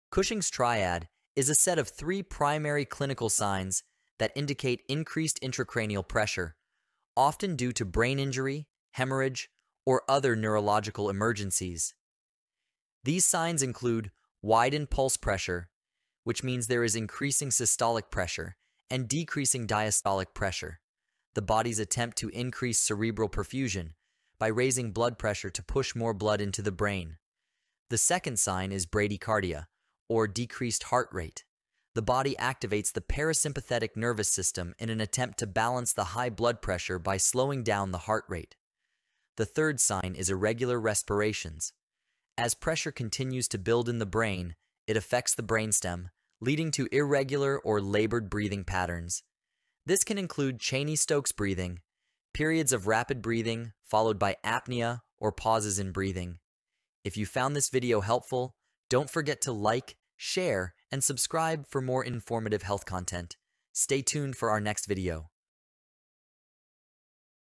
Cushing's Triad of Raised Intracranial Pressure (ICP) | Cushing Reflex | Cushing Phenomenon
Cushing's Triad is a set of three clinical signs that indicate increased intracranial pressure, often due to brain injury or hemorrhage. These signs include widened pulse pressure (with increasing systolic and decreasing diastolic pressure) as the body attempts to increase blood flow to the brain, bradycardia (a slowed heart rate) due to parasympathetic activation in response to high blood pressure, and irregular respirations caused by brainstem compression, often leading to abnormal breathing patterns like Cheyne-Stokes. These signs signal a medical emergency requiring immediate attention.